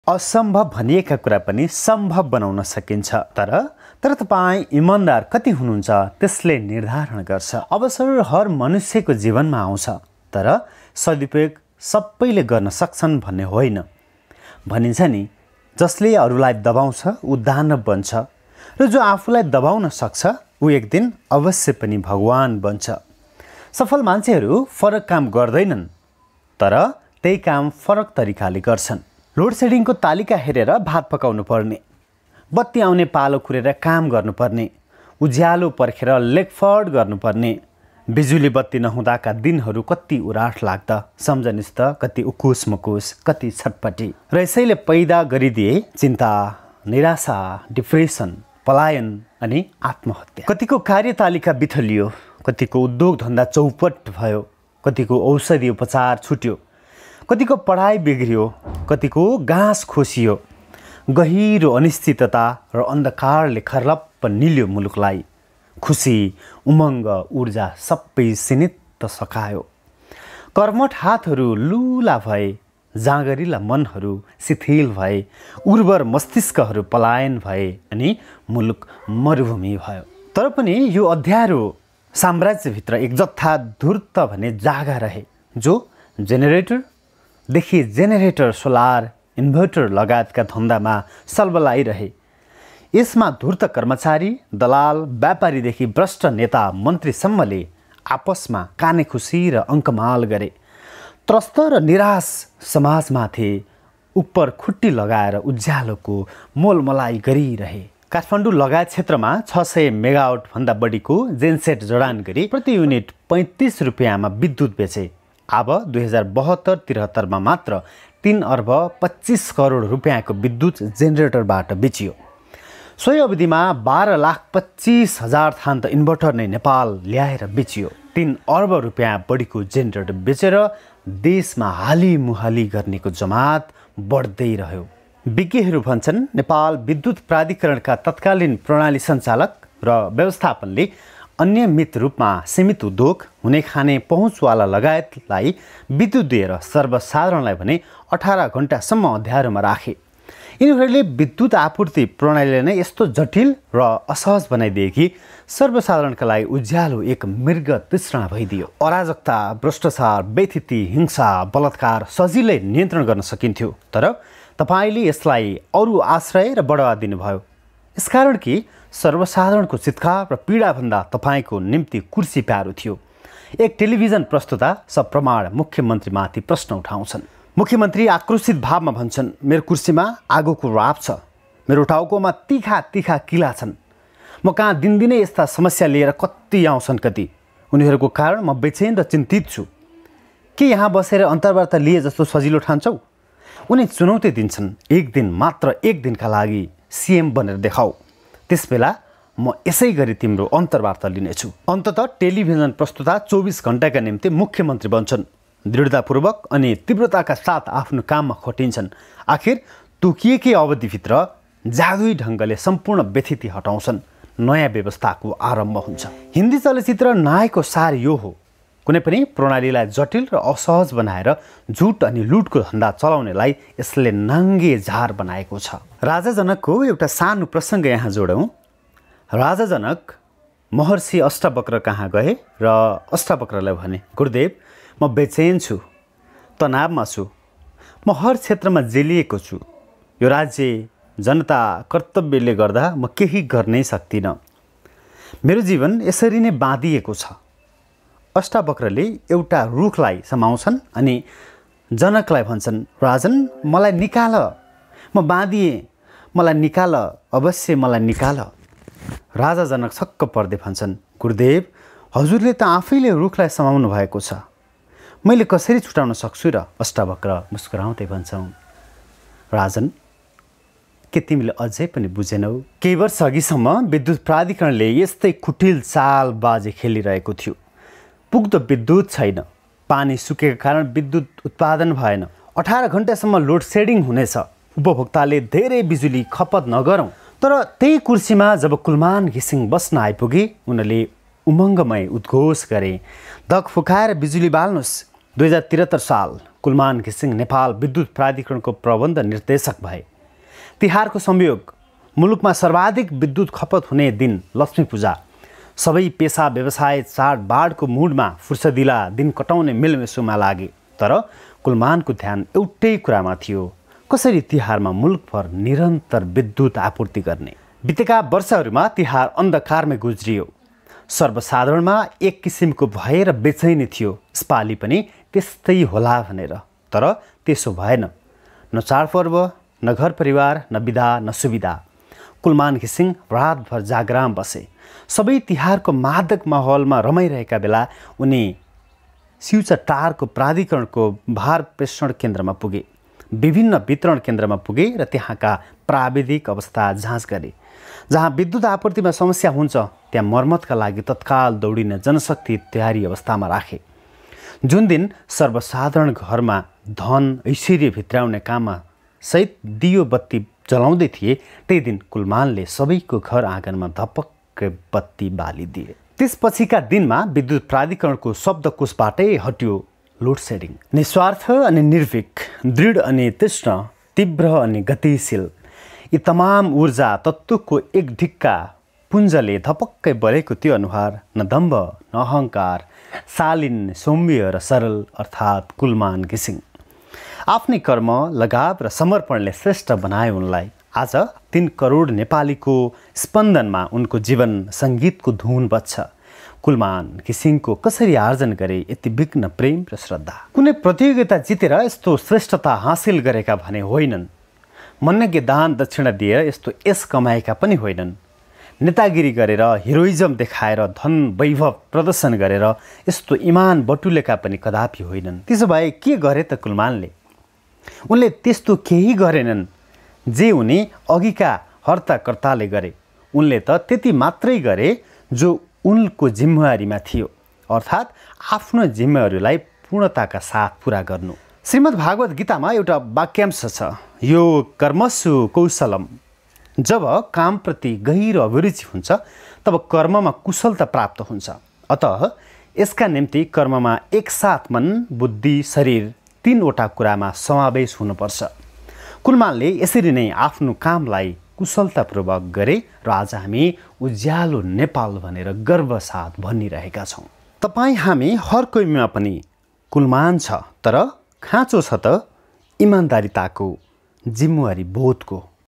कुलमान हामी जोकोही भित्र छ | Kulman Ghising The Hero of Nepal
नेपाल केहिबर्ष अघिसम्म लोडसेडिङको मारले थिलथिलो थियो । लोडसेडिङको तालिका हेरेर हर काम गर्नुपर्ने हुन्थ्यो । बत्ती नहुँदाका दिनहरु कति उराठलाग्दा, उकुसमुकुस र कति छटपटीका थिए । भरदिन बत्ति आउने त नेपालीको एकादेशको कथा थियो । तर आज २४ सै घण्टा बिजुली बत्तिको पहुँचमा नेपाल छ । तर यो काम सजिलो पक्कै थिएन । तर एक कर्मठ, इमानदार र जिम्मेवार ब्यक्तिकै अठोटमा आज हामीले 'उज्यालो नेपाल' देख्न पाएका छौं ।  चिन्ता, निरासा, डिप्रेसन, पलायन अनि आत्महत्या जस्ता अनेकौं समस्याबाट एक कदम माथि उठाउने ती व्यक्ति कुलमान घिसिङ हुन् । कुलमानले झै अन्य देशभक्तको पनि अब खाचो छ ।
यस च्यानल मा रहेका Video हरु कपि, Download गरी अन्य Channel मा  गरेको पाएमा प्रचलित कानुन बमोजिम कारबाही गरिनेछ